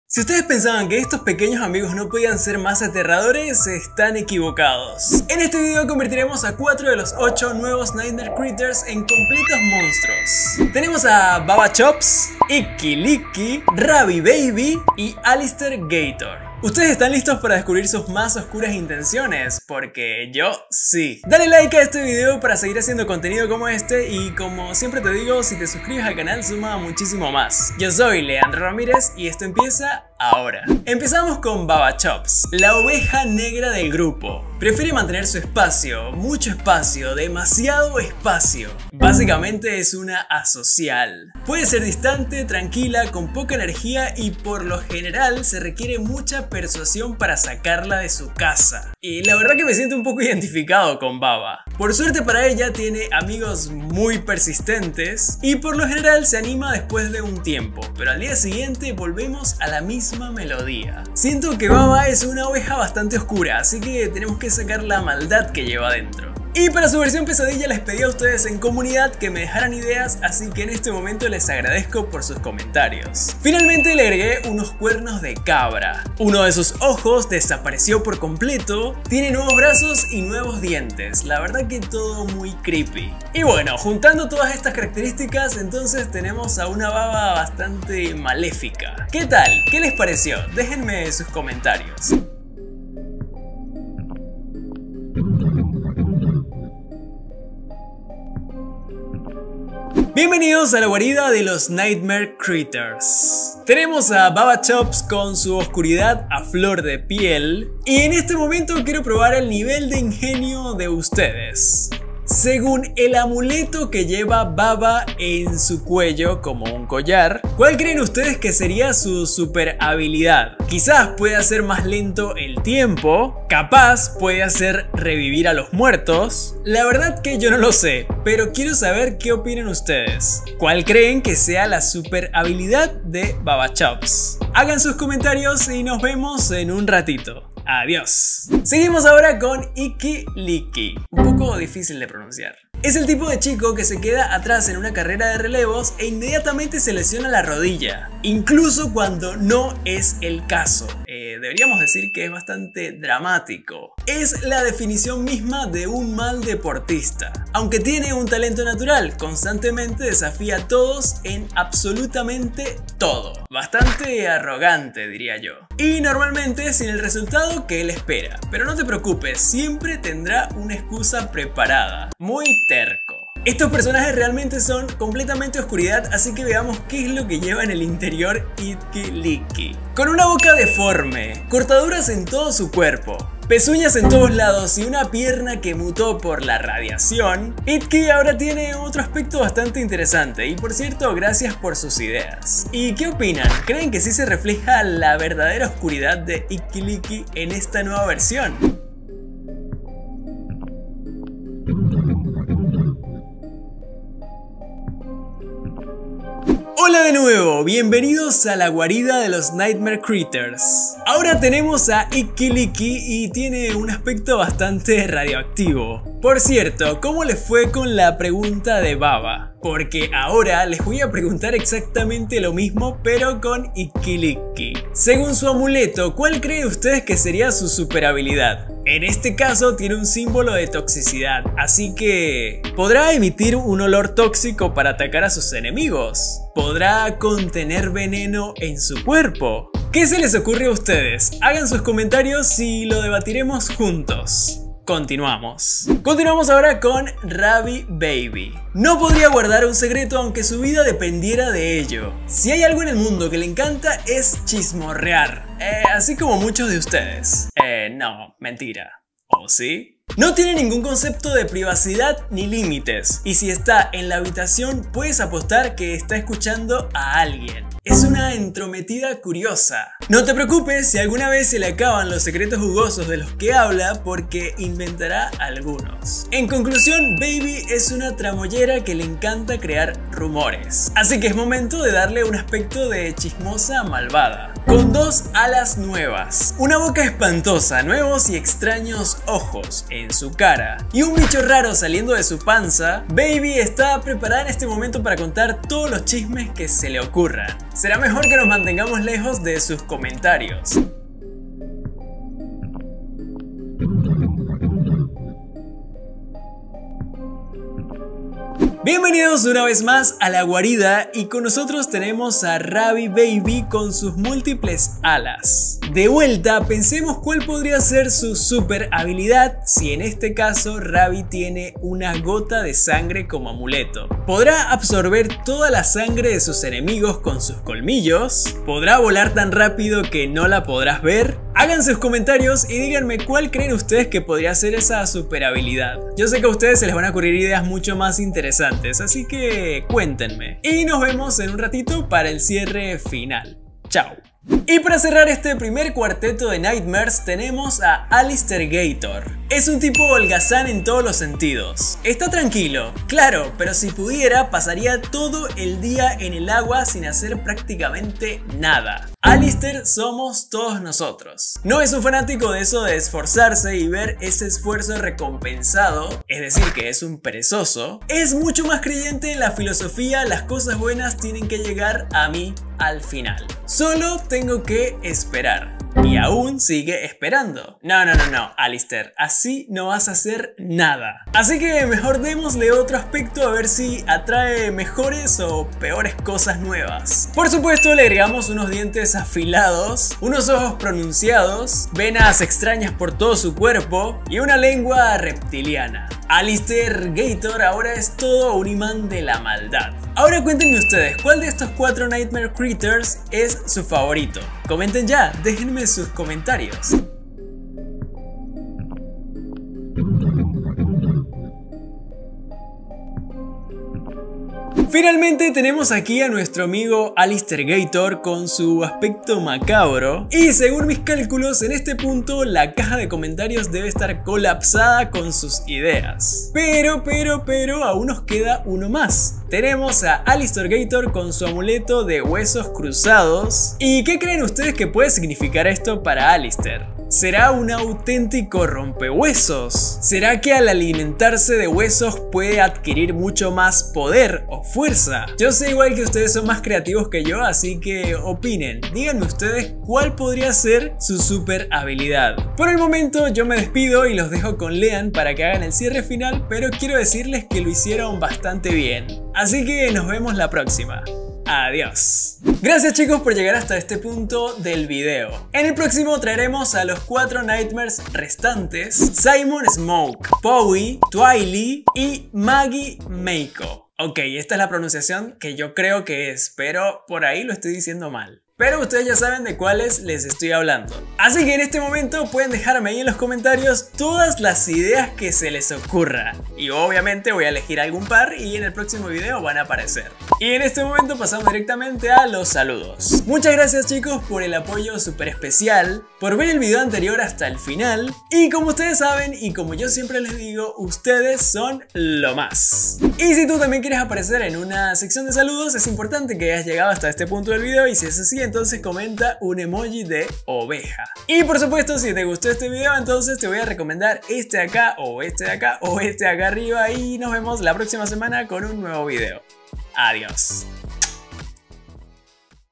POPPY PLAYTIME 4 Nightmare Critters MONSTRUOS 😱🔥
Hola CHICXS!!! 🔥 - En este video dibujaremos y convertiremos a BABA CHOPS, ICKY LICKY, RABIE BABY y ALLISTER GATOR (Los nuevos NIGHTAMTE CRITTERS de Poppy Playtime) en completos Monstruos, espero les guste!
Salud2s!! 🔥